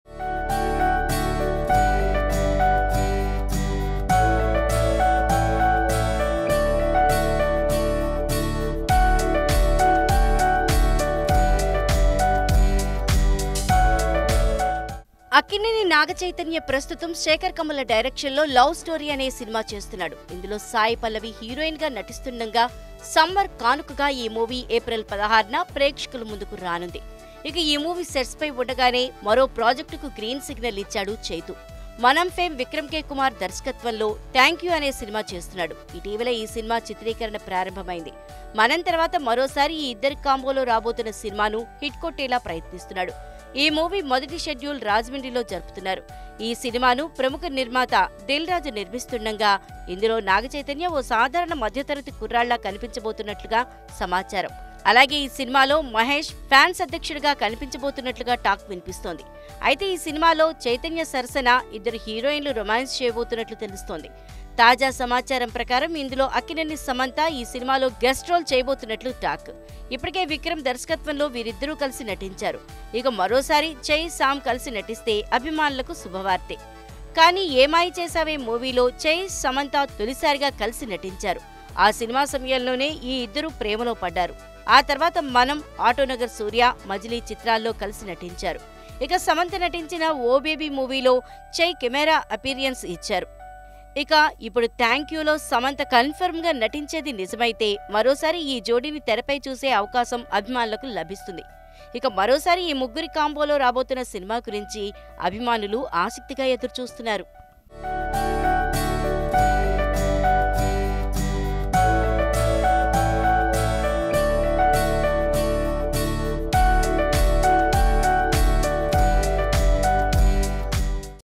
After Majili, speculations run high on Naga Chaitanya - Samantha reunion in "Thank You" - TV9
After Majili, speculations run high on Naga Chaitanya - Samantha reunion in "Thank You"

Credit: Tollywood | Naveena /Producer | Veera /Editor